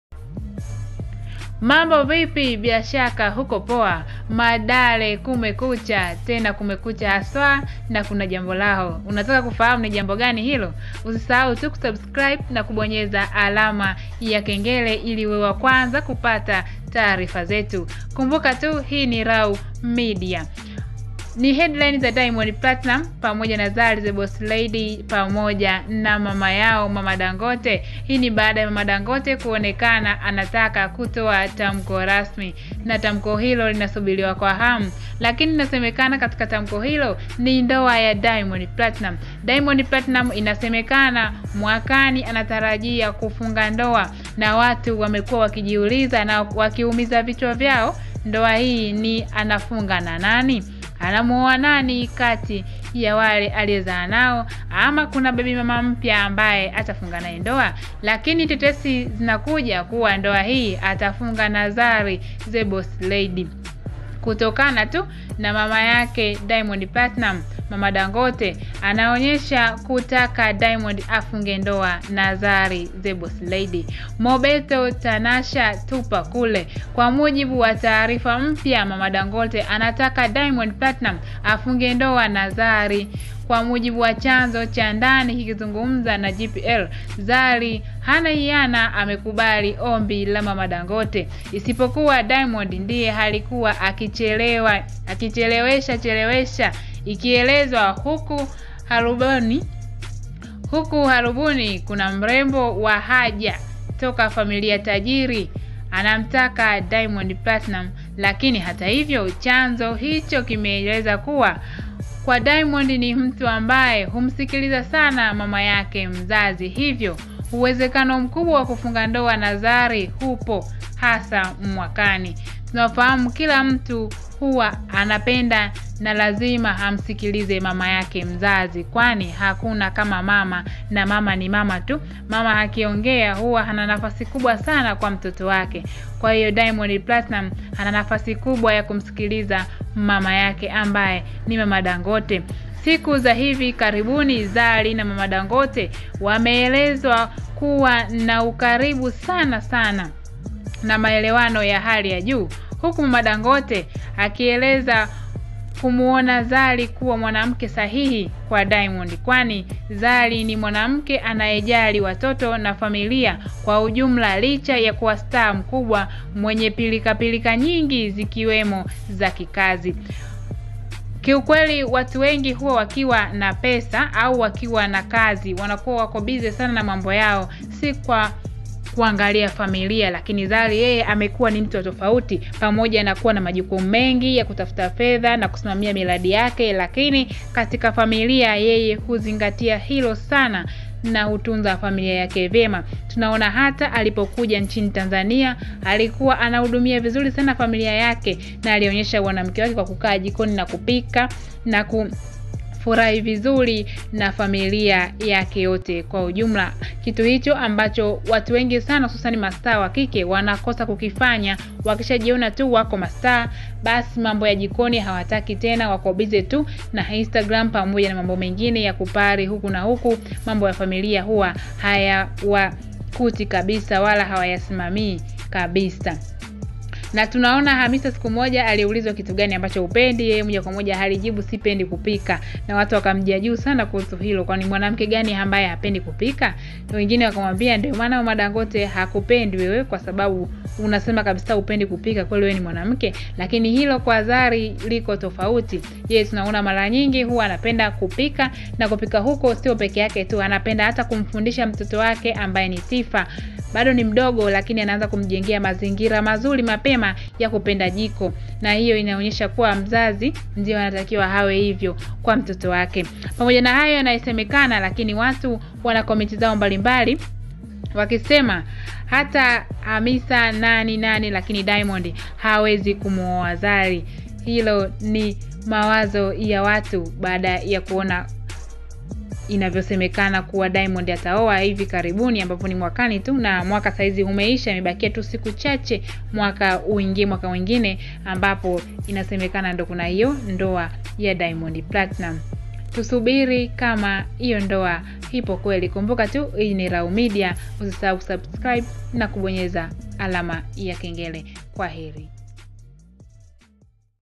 Kumekucha ndoa ya Diamond na Zari yawadia / hamisa tanasha tupa kule
Kumekucha ndoa ya Diamond  na Zari  yawadia / hamisa tanasha tupa k